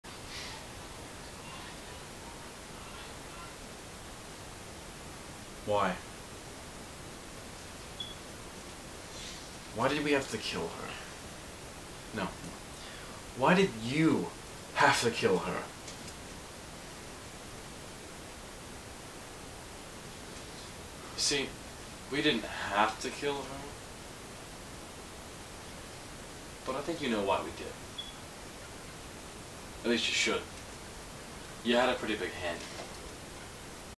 -I Think You Know Why- |Short
Inspired by a scene from the show Gotham. However, this is mainly just an editing test, do not take it seriously.